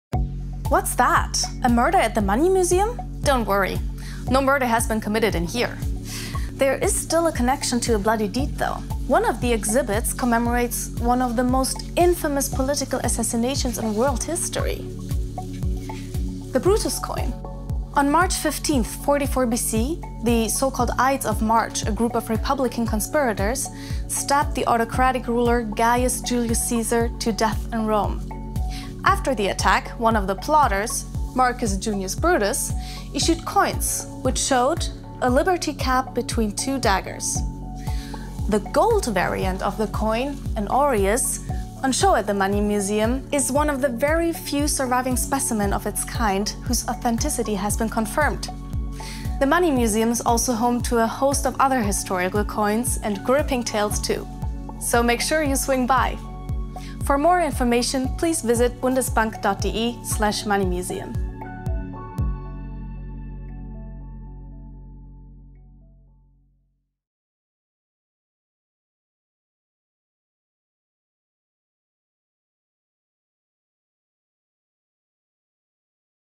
Ever heard of the golden Brutus coin?
The Ides of March aureus is one of the rarest and therefore most valuable Roman coins in the world.  One of these is housed in the British Museum and another in the Bundesbank's Money Museum. In the first part of our short video series showing highlights from the Money Museum, presenter Silviana Ursu explains the story behind this special coin.

Adress:
Deutsche Bundesbank
Wilhelm-Epstein-Straße 14
60431 Frankfurt am Main Socials: